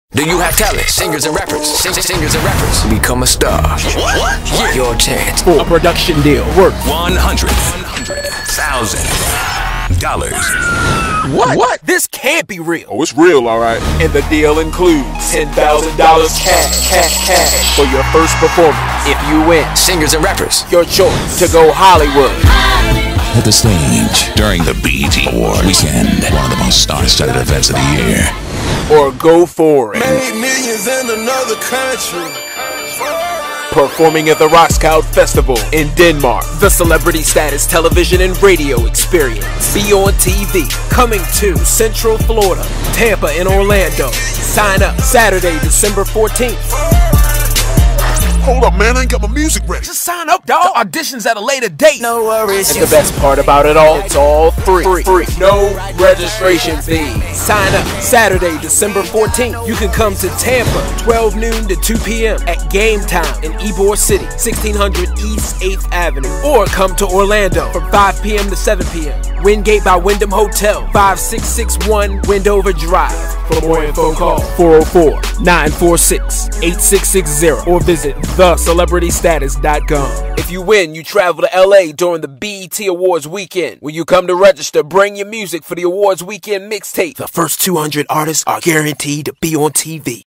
The Celebrity Status - Central Florida Showcase Registration Event
Artists are you ready to take your careers to the next level? Is your talent worth $100,000? Come out and see me at the registration event. You will not need to have your performance ready but you will need to attend in order to enter the showcase. At the registration events you will be given the date, time, location and learn what the judges will be looking for. Tell em Jefe sent cha!!